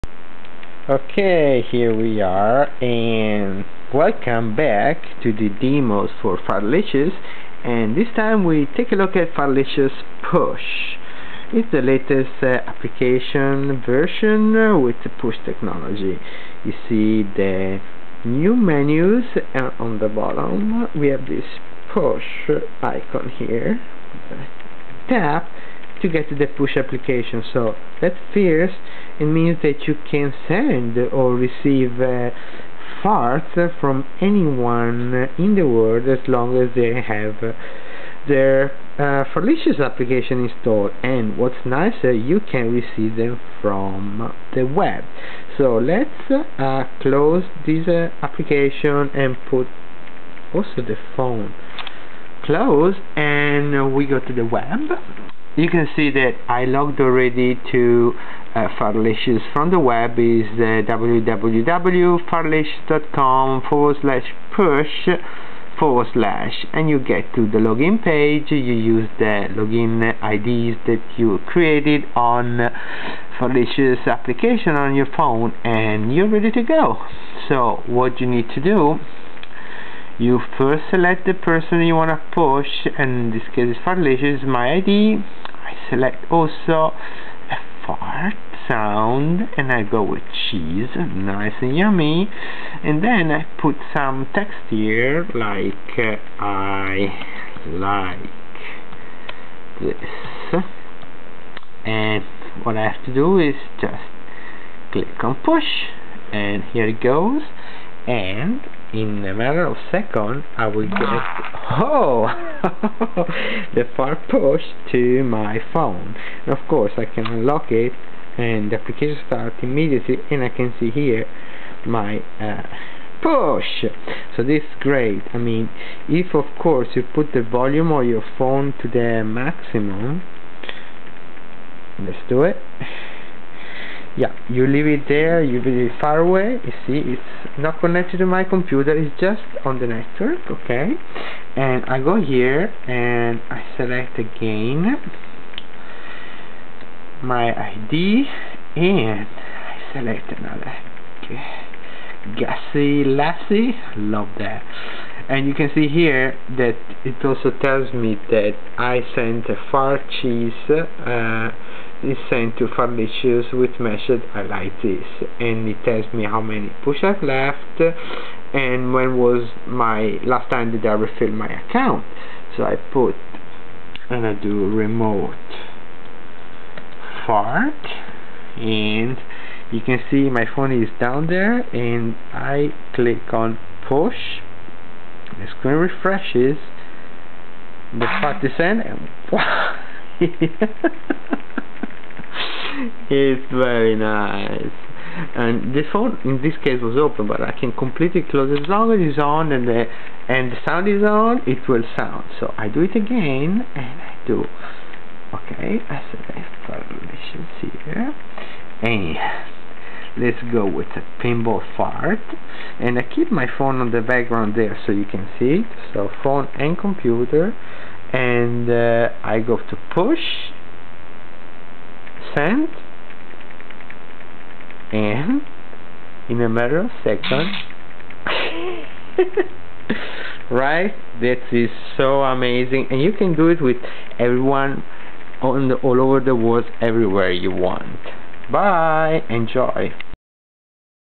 Fart push Demo
With version 4.0 (4.1) farts can be pushed from your phone or the web to other iPhones or iPod touch with Fartlicious installed.